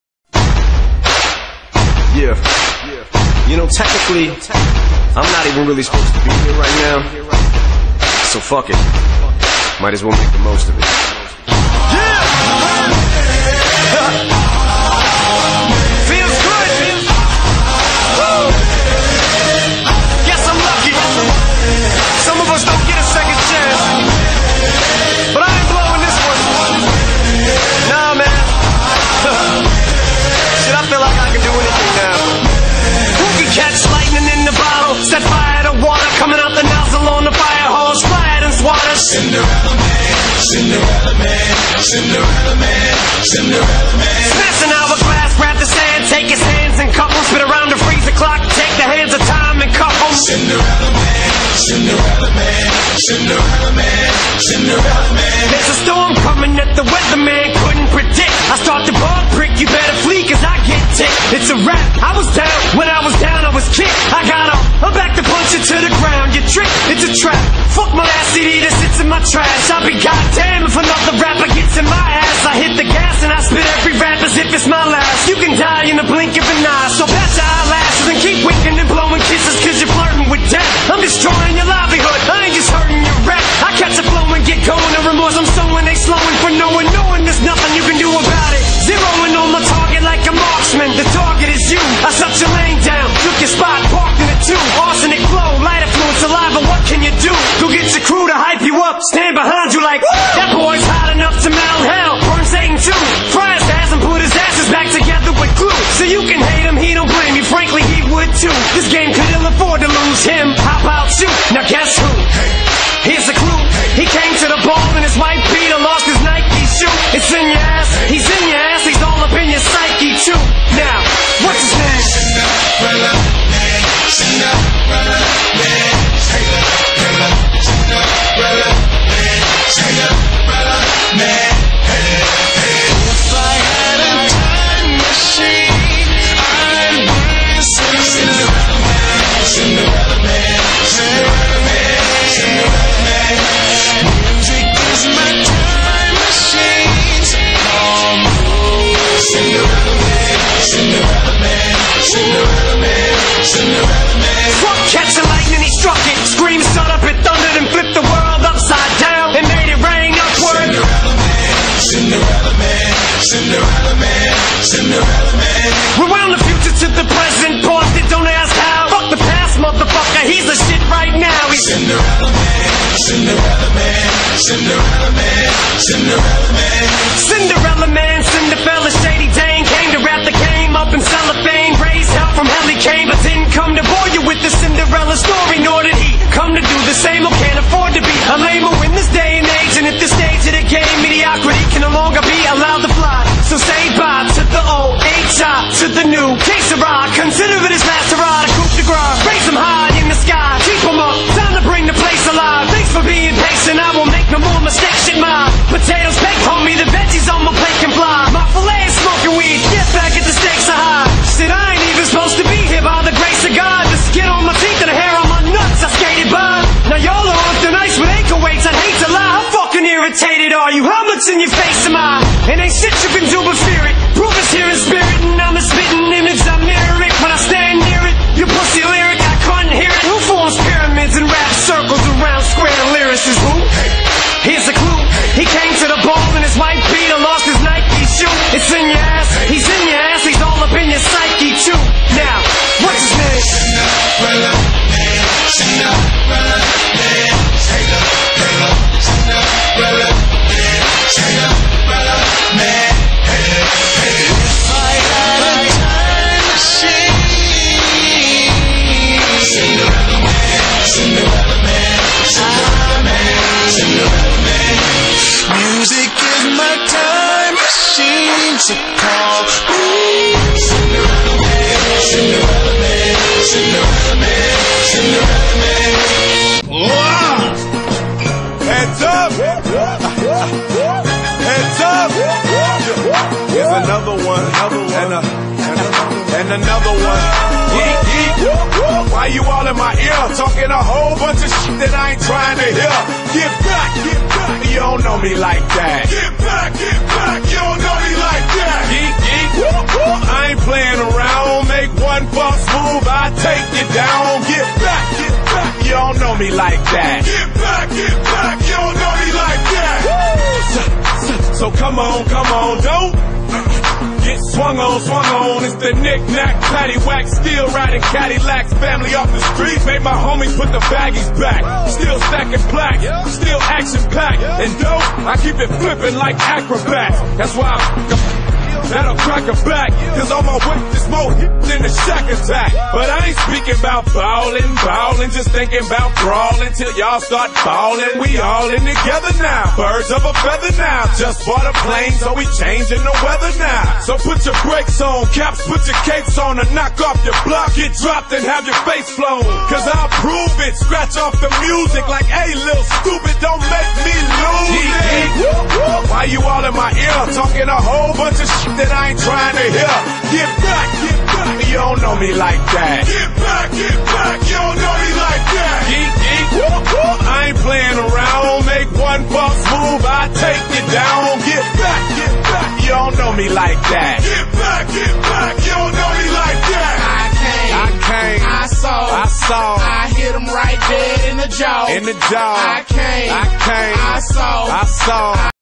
Marc Schiechl Senior Year Highlights
Marc Schiechl Senior Year Defensive End #94 Footage From 8 Games